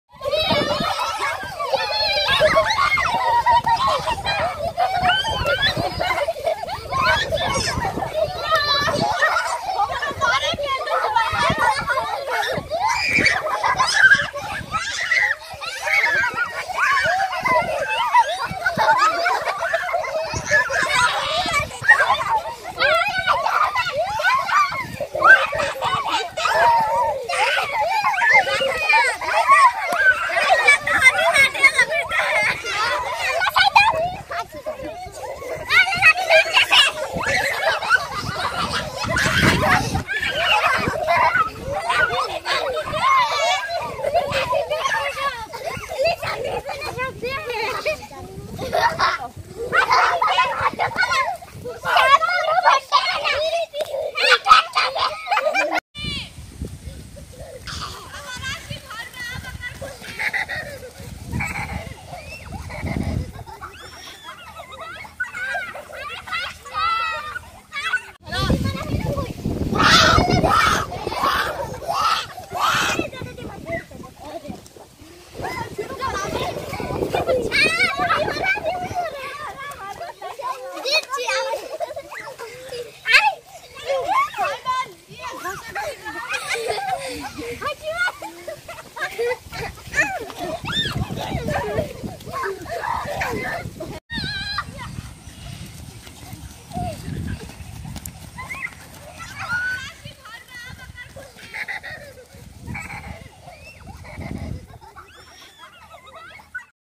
কানামাছি ভোঁ ভোঁ, ছোটদের হারিয়ে যাওয়া গ্রাম বাংলার ঐতিয্য। দৌড় প্রতিযোগীতা ও মুরগী লড়াই।
"আজকের ভিডিওতে দেখুন গ্রাম বাংলার হারিয়ে যাওয়া ঐতিহ্যগুলো! কানামাছি ভোঁ ভোঁ খেলা, দৌড় প্রতিযোগীতা, এবং মুরগী লড়াই—এসব আজকের প্রজন্মের কাছে প্রায় অজানা। এই ভিডিওতে আমরা ফিরে যাব সেই সময়ের মধ্যে, যখন এসব খেলা গ্রামের মানুষদের আনন্দের অংশ ছিল। ছোটদের জন্য মজাদার, উত্তেজনাপূর্ণ এবং পুরনো দিনের গ্রামবাংলার সৌন্দর্য! দেখে নিন কিভাবে এই ঐতিহ্যগুলো আজও আমাদের মধ্যে ছড়িয়ে পড়েছে।"